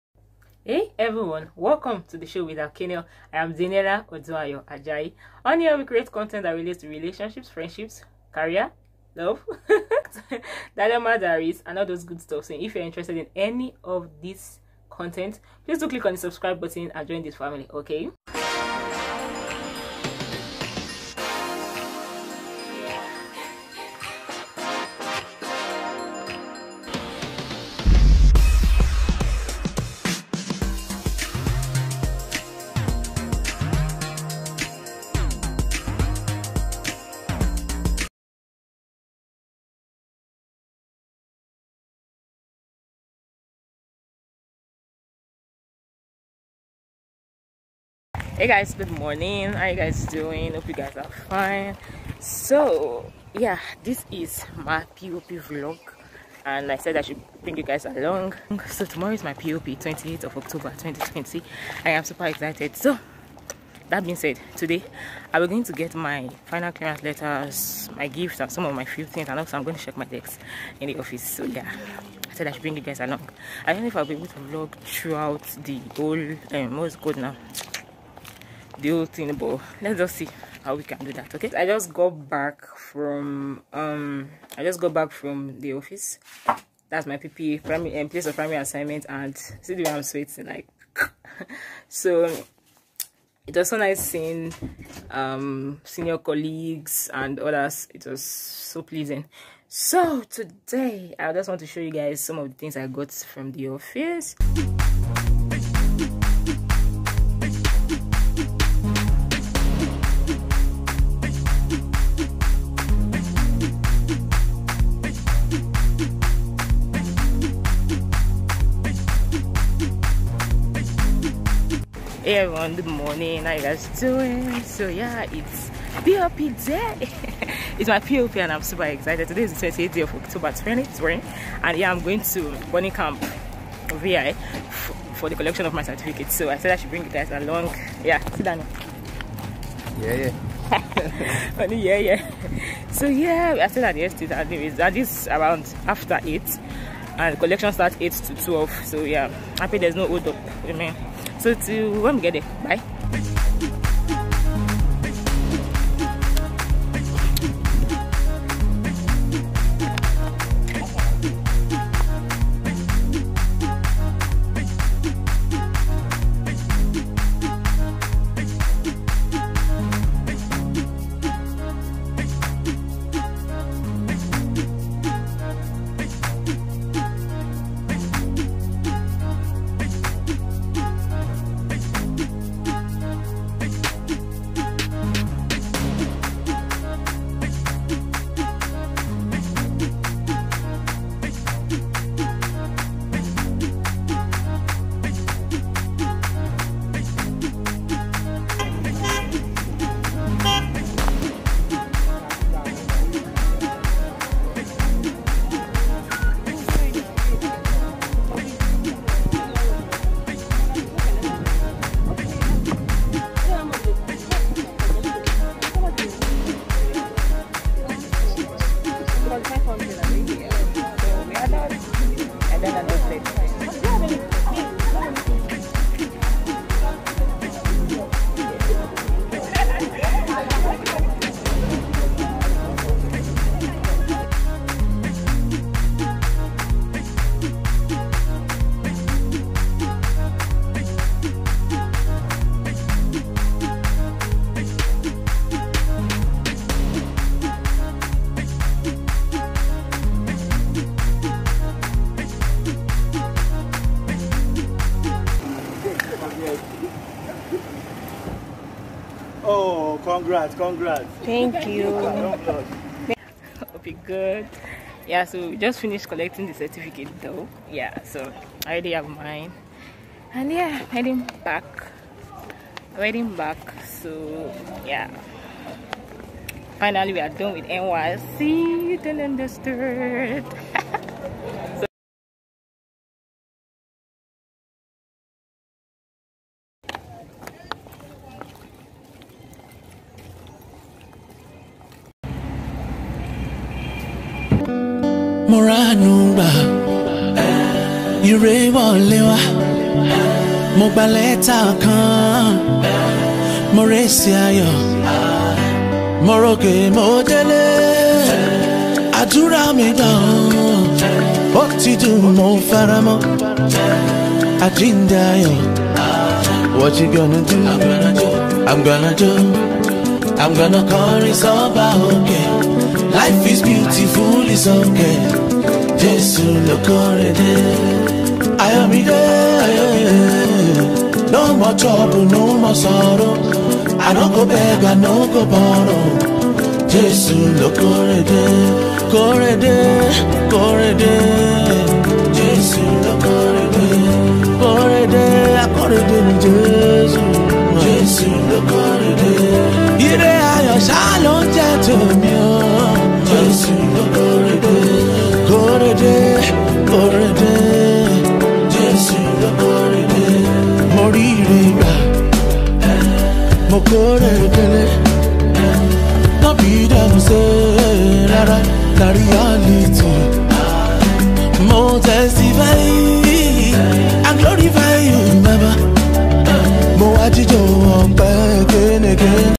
MY PASSING OUT PARADE VLOG (P.O.P)VLOG💃💃💃
I concluded my NYSC on the 28th Day of October 2020.